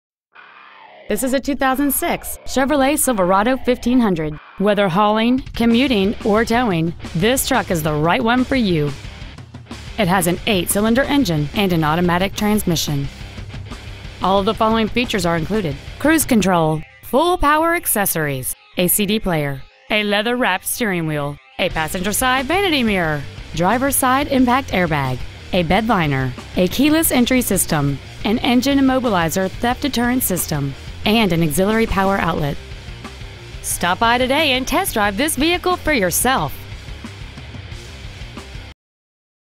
2006 Chevrolet Silverado 1500 New Orleans LA 70126
Year : 2006
 Make : Chevrolet
 Model : Silverado 1500

 Trans . : Automatic
 Exterior : BLUE
 Miles : 97,170
 Interior : GRAY

 Used 2006 Chevrolet Silverado 1500 New Orleans LA